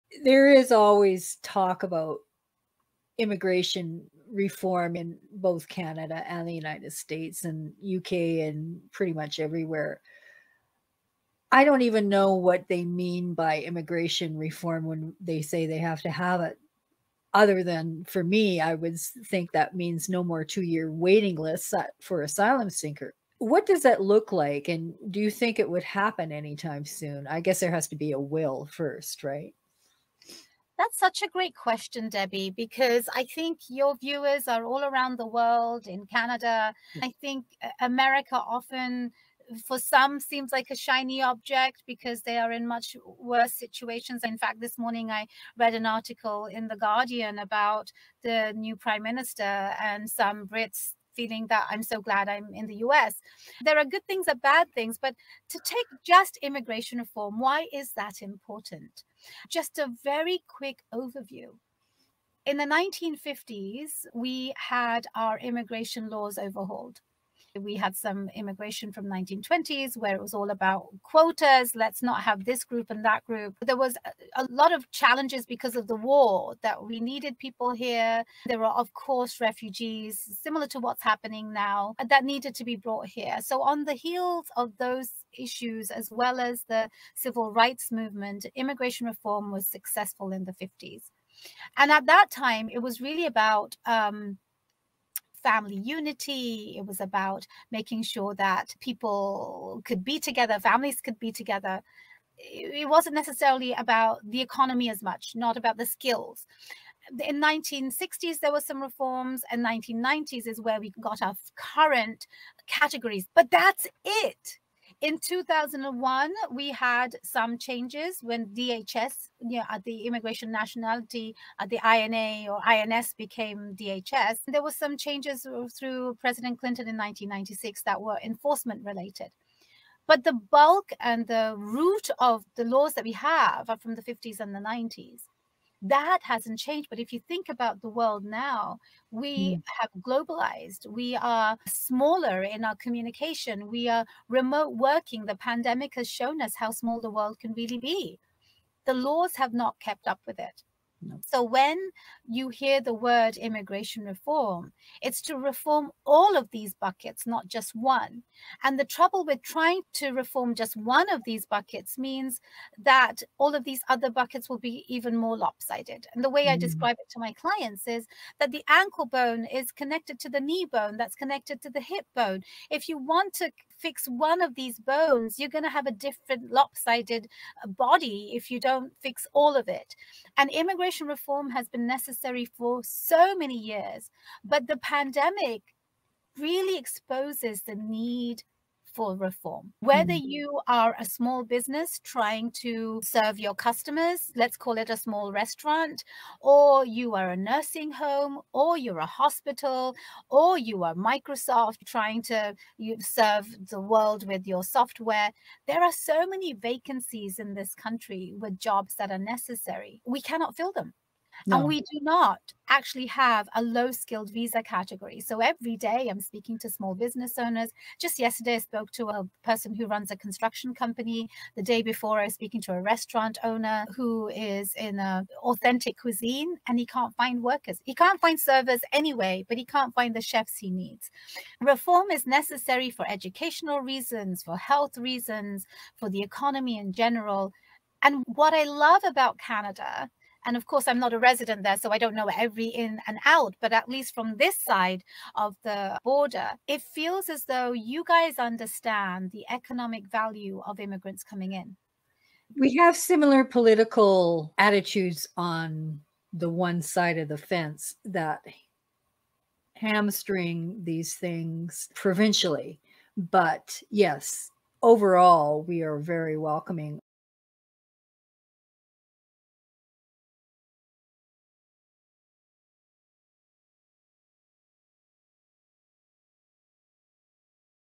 Immigration Is Desperately Needed for the Economy
Tahmina Watson shares why immigration is desperately needed. She speaks to local businesses who can't find workers.